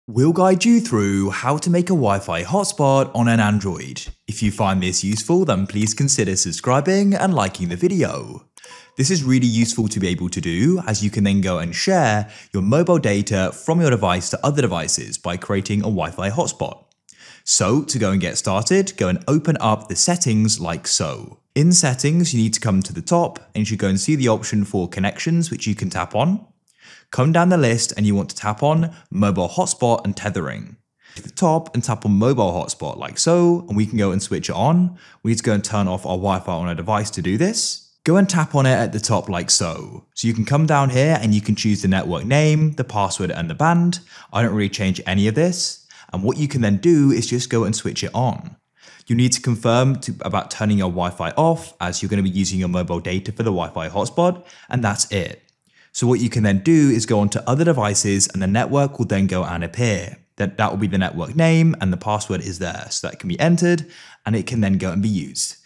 How To Make WiFi Hotspot On Android - Full Guide
Learn how to make wifi hotspot on android in this video.

GuideRealm